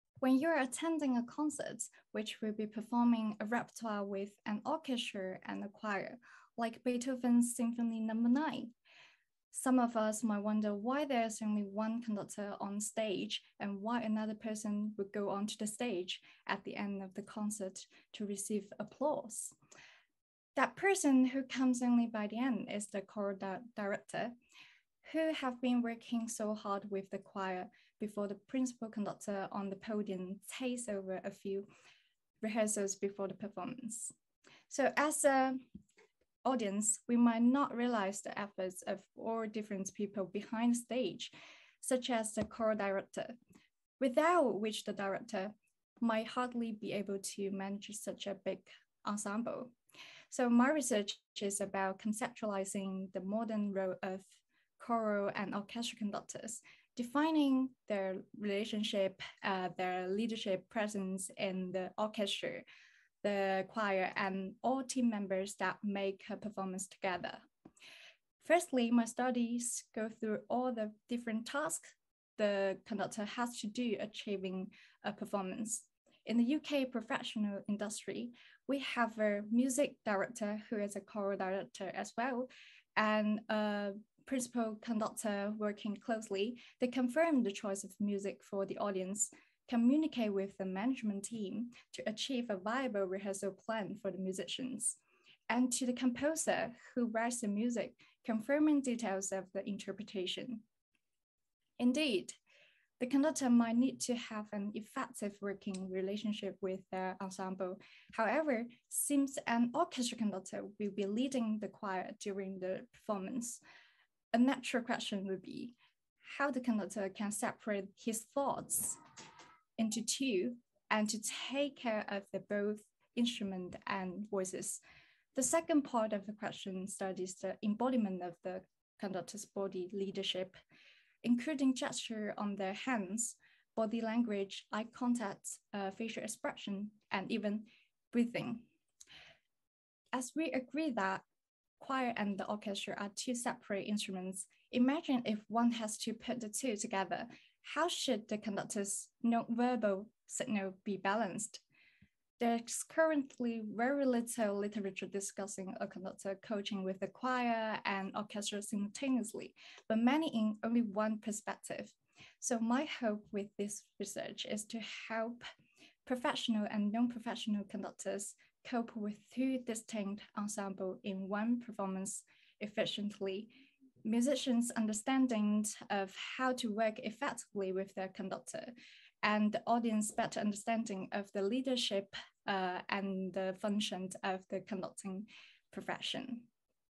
3MT 2022 - Yajie Ye - Arts - Leadership presence: Choral-orchestral conductor
The choral-orchestral conductor’s leadership principles of shaping a successful ensemble are arguably equally desirable in many nations, including both the UK and China. This research explores choral strategies in western symphonic repertories and their localisation in China from 1976 to the present. I am investigating how the relationship between choral and orchestral conducting has been conceptualised in the UK. This also seeks to shape an understanding of the symbiotic network between ensemble and conductor.